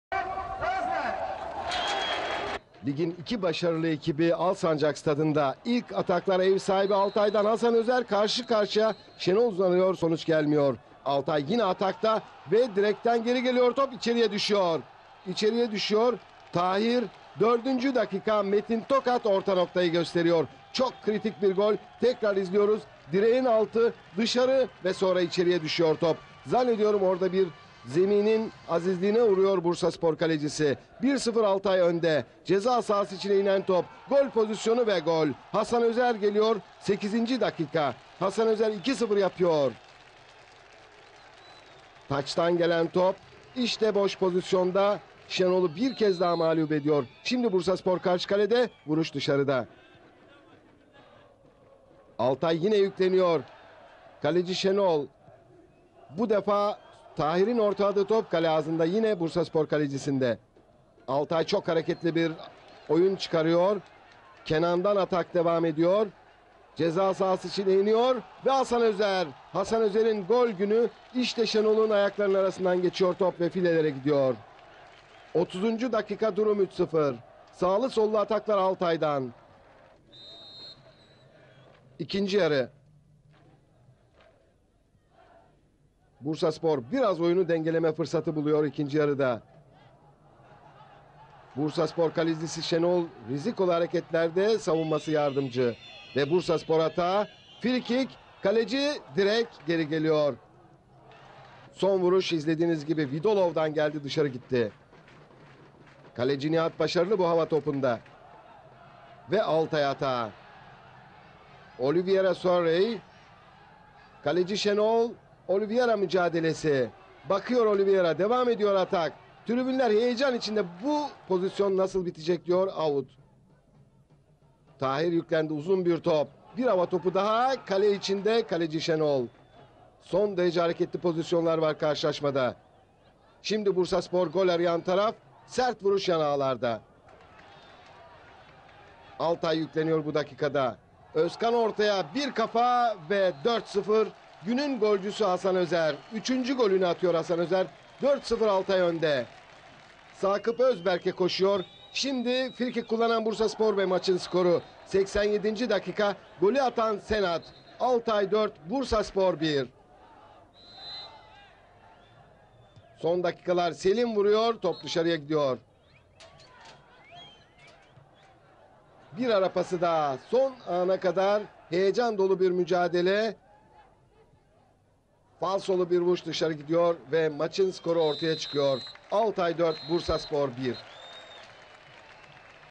BÜYÜK ALTAY - BURSASPOR 1997 - 1998 SEZONU 9.HAFTA MAÇI
Altay 5 puanla  küme düşme hattının bir basamak üzerinde ancak Samsunspor  küme düşme hattında ve 5 puana sahip.  Bursapor ise 11 puanla 11.sırada yer alıyor.
Büyük Altay ise galibiyetten başka bir şey düşünmüyor.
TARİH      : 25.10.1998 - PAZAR
SAAT        : 14.00
STAD       : ALSANCAK  STADI
HAKEM   : METİN TOKAT

BÜYÜK ALTAY KADROSU
Teknik Direktör  : SAKIP ÖZBERK
İlk 11
1. NİHAT TÜMKAYA
2. MURAT ALAÇAYIR
3. TAHİR KARAPINAR
4. ORHAN ÜSTÜNDAĞ
5. EMMANUEL E.TEBEREN
6. KENAN ARAYICI
7. SERKAN DÖKME
8. ÖZKAN KOÇTÜRK
9. PASCAL PATRICK
10. OLIVIER MARCEL GHISLAIN SURAY
11. HASAN ÖZER
 Yedekler
12. AYDIN ETHEM DAĞDELEN
13. ALİ KEMAL ALATAŞ
14. CÜNEYT YİS
15. EMRE GÜSAR
16. İBRAHİM BOHARI
17. SERKAN KARABABA
18. ŞENOL İBİŞİ
 Goller
  TAHİR KARAPINAR,3.dk (F)
  HASAN ÖZER,8.dk (F)
  HASAN ÖZER,29.dk (F)
  HASAN ÖZER,82.dk (F)
 Kartlar
Sarı Kart ORHAN ÜSTÜNDAĞ 14.dk
Sarı Kart NİHAT TÜMKAYA 74.dk
Sarı Kart OLIVIER MARCEL GHISLAIN SURAY 7.dk
 Oyundan Çıkanlar
 ORHAN ÜSTÜNDAĞ 80.dk
 PASCAL PATRICK 70.dk
 OLIVIER MARCEL GHISLAIN SURAY 70.dk
 Oyuna Girenler
EMRE GÜSAR 80.dk
İBRAHİM BOHARI 70.dk
CÜNEYT YİS 70.dk